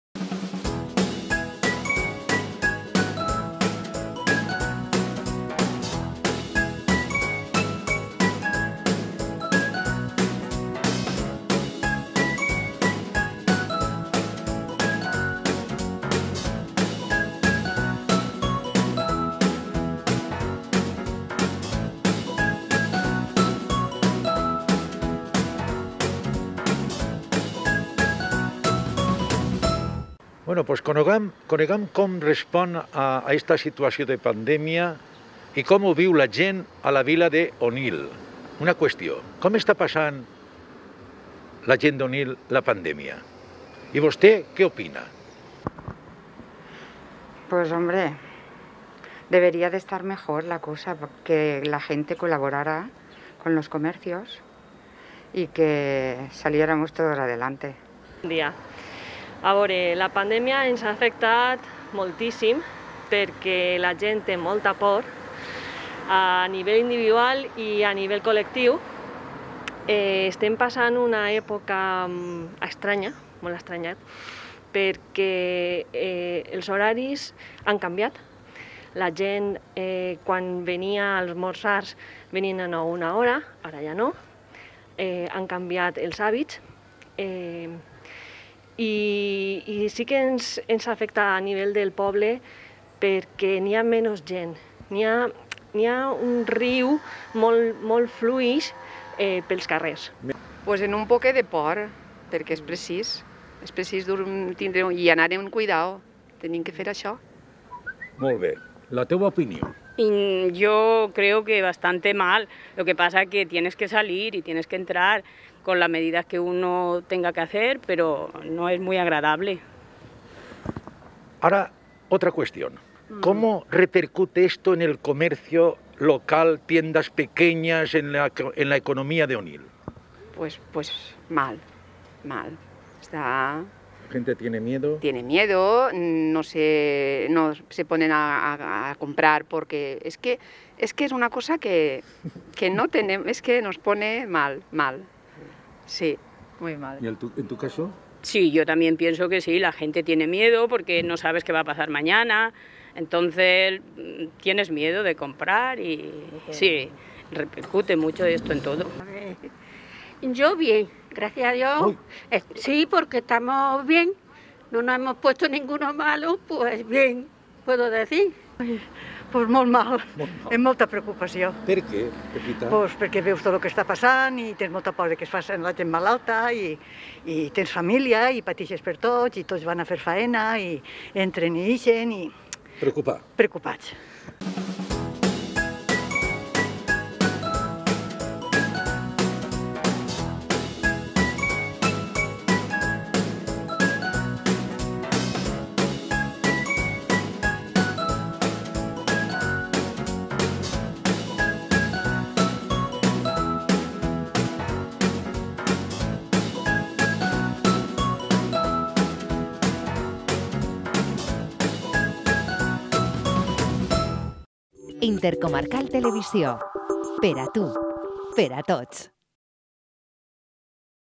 Onil: Com està vivint la crisi de la Covid-19?
Hem visitat Onil per a conéixer l'opinió dels seus ciutadans i ciutadanes En esta ocasió volem saber com estan vivint la crisi de la Covid-19.